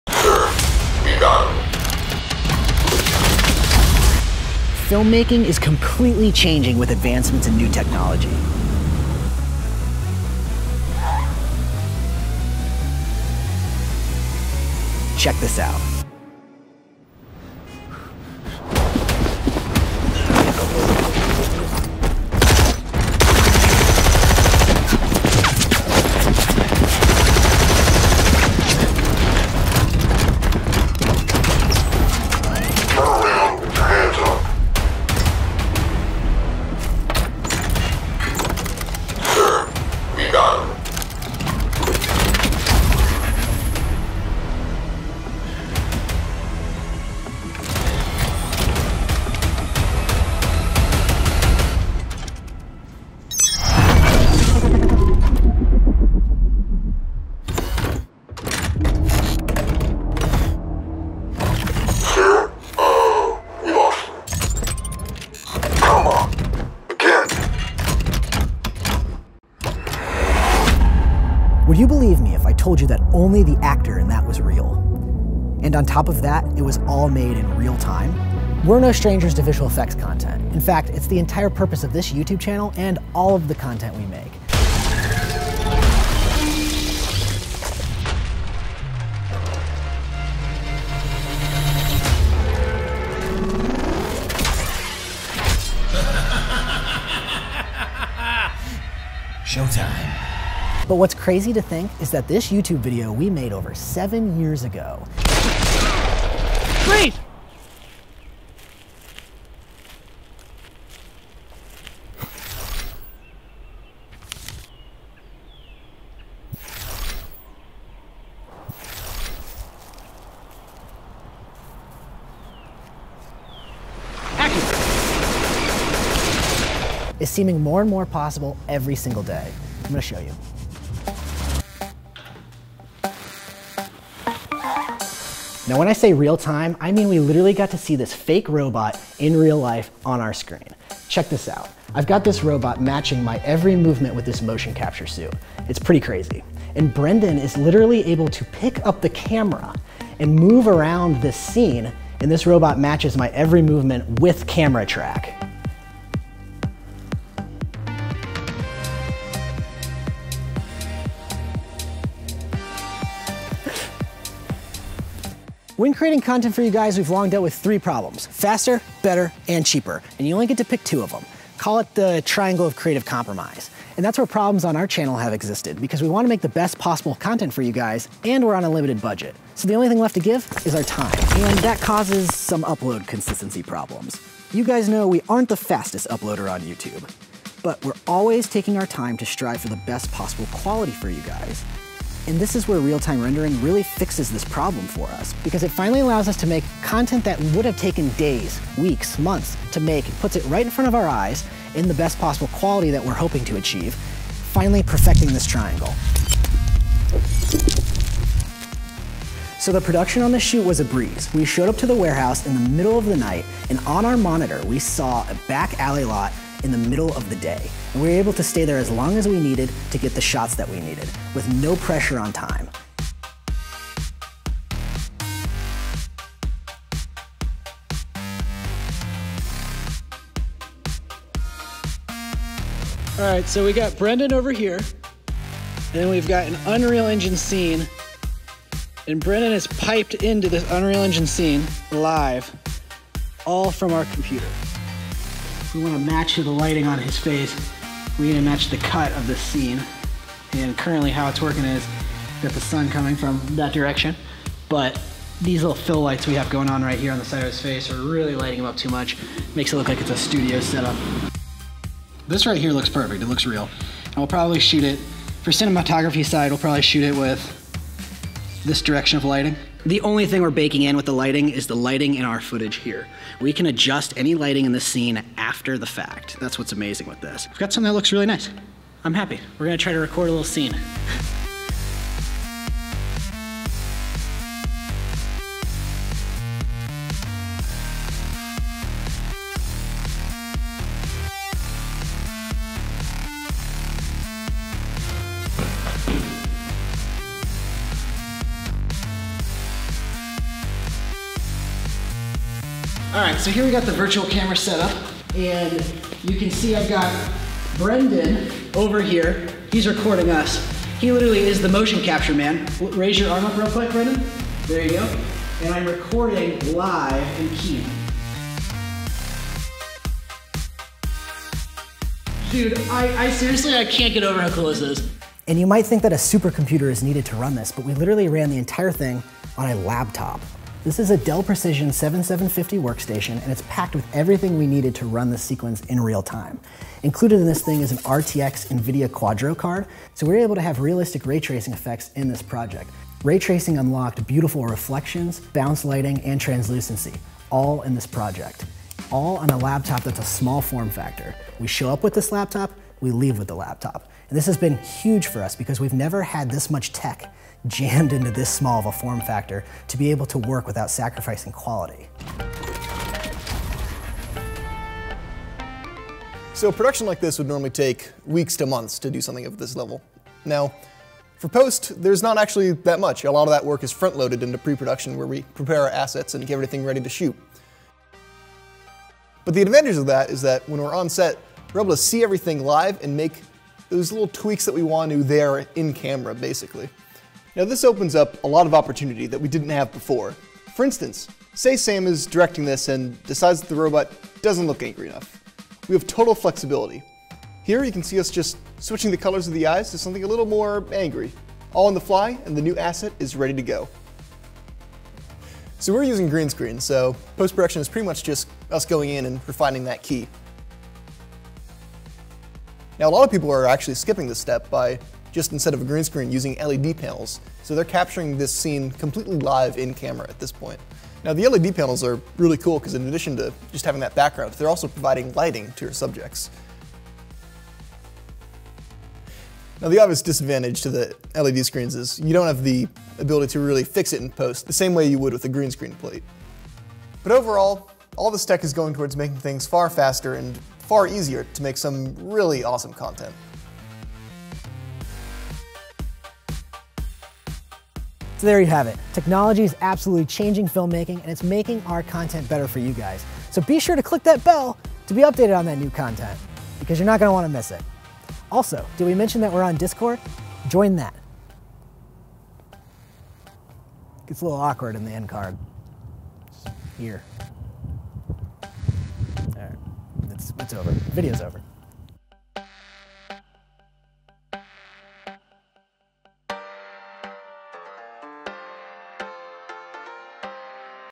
The Future of CGi
Check out a sneak film we made as we take a look at some incredible new technology that is changing the way we create films. Virtual production, CGI and new VFX techniques are finally making it cheaper, faster, and easier than ever before to make incredible films!

We’re huge fans of all the tech we use so check out the links below to support these incredible companies making this technology possible:

Special Thanks to VFX Technologies and Stargate Studios in Los Angeles for providing LED Panel footage to use in this video.

Sound Design by Chris Delgado